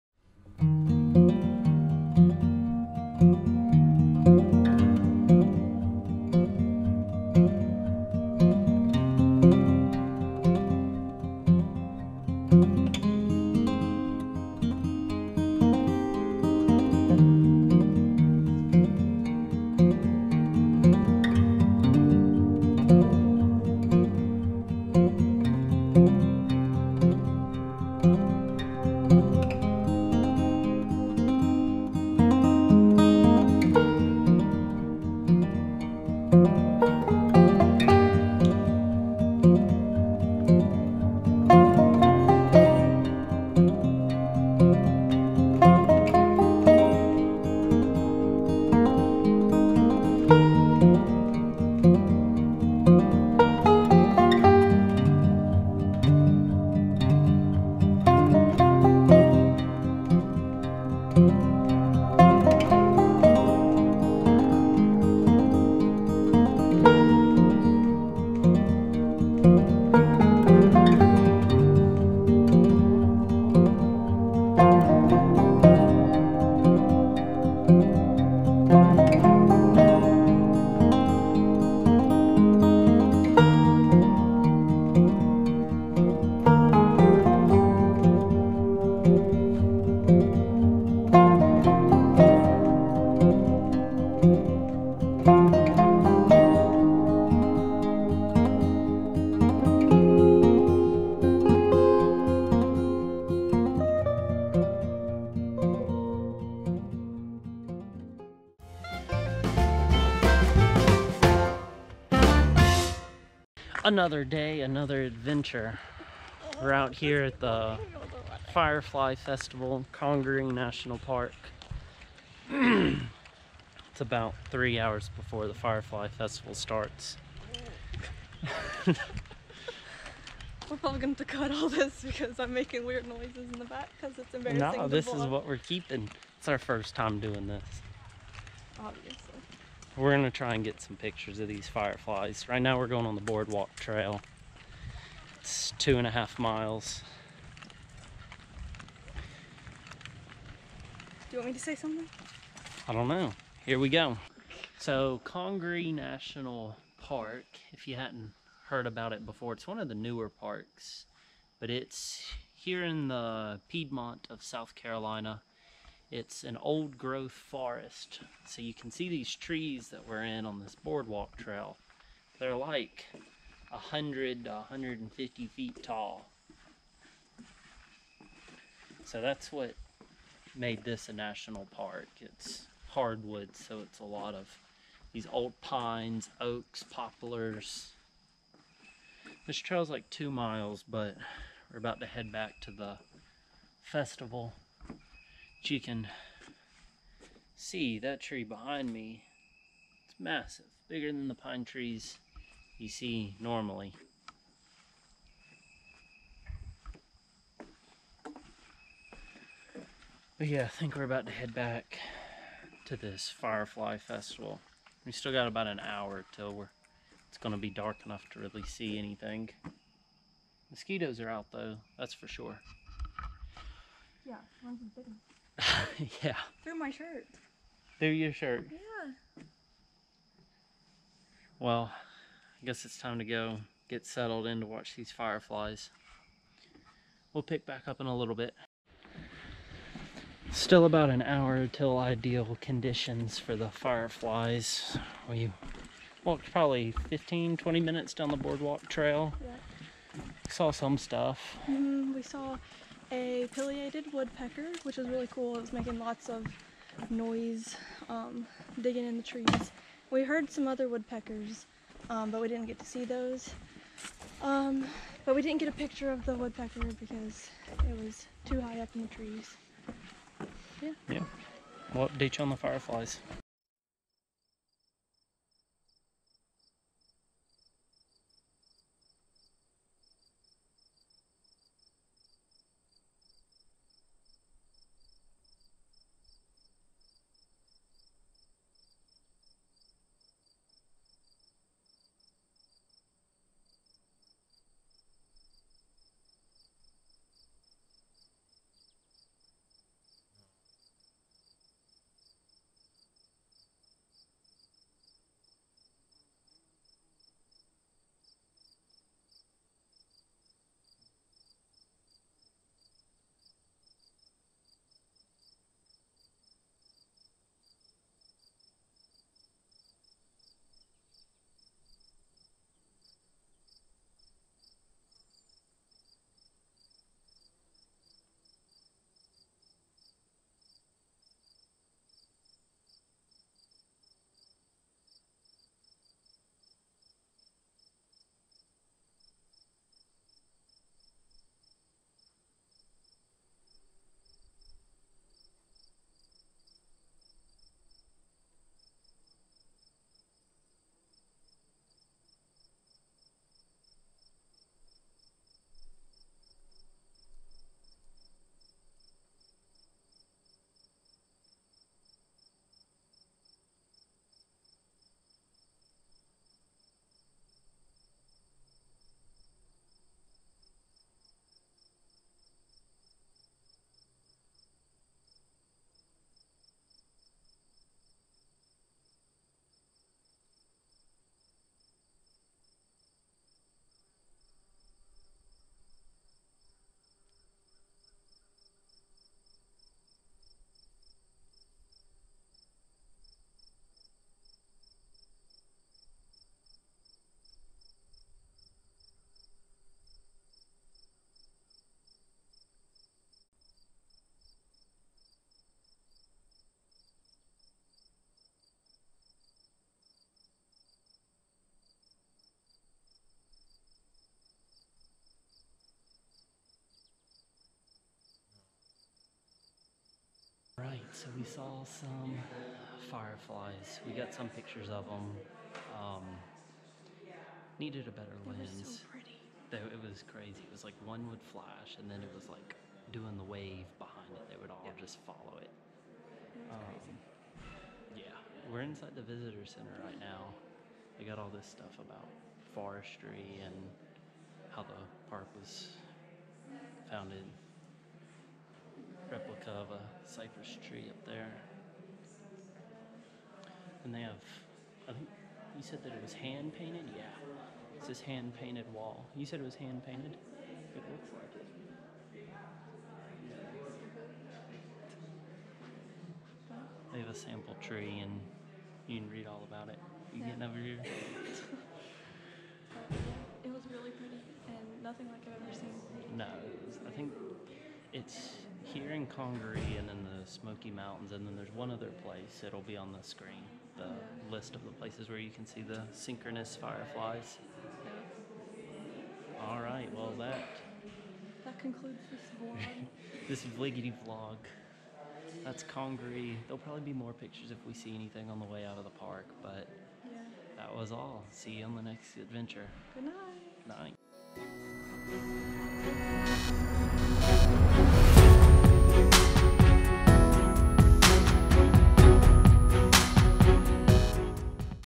Congaree National Park Firefly Festival 2019
May 2019 Firefly Festival at Congaree National Park. We drove down for the weekend to check out the Firefly Display at Congaree. This was an amazing opportunity and a lifetime experience.

FOLLOW ALONG!!